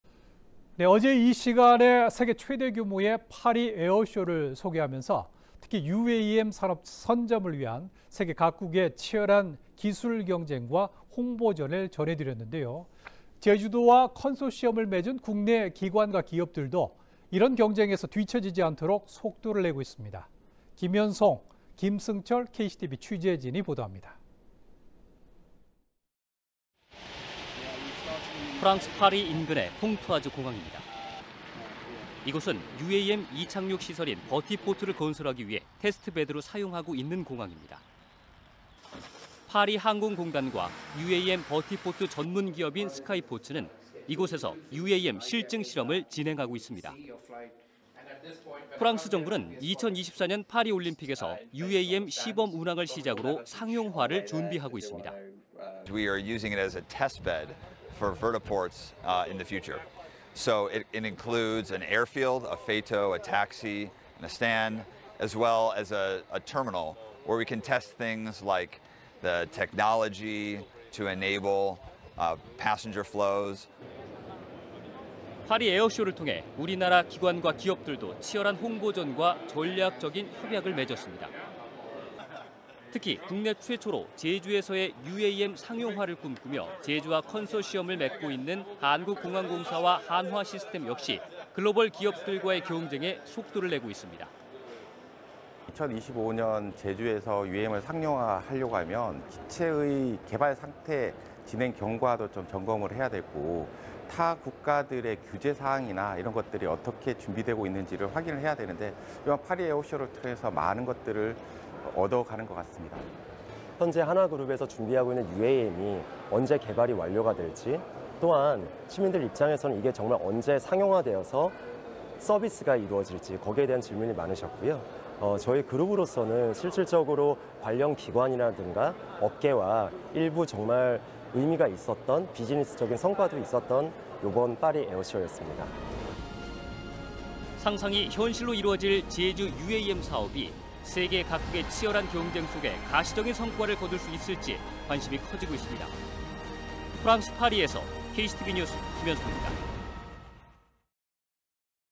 2023년 파리에어쇼② 제주 UAM "속도 낸다"
어제 이 시간에 세계 최대규모의 파리에어쇼를 소개하면서 특히 UAM 산업 선점을 위한 세계 각국의 치열한 기술경쟁과 홍보전을 전해드렸는데요.

현장에서는 제주도와 컨소시엄을 맺고 있는 국내 기관과 기업들도 세계 경쟁에서 뒤쳐지지 않도록 속도를 내고 있습니다.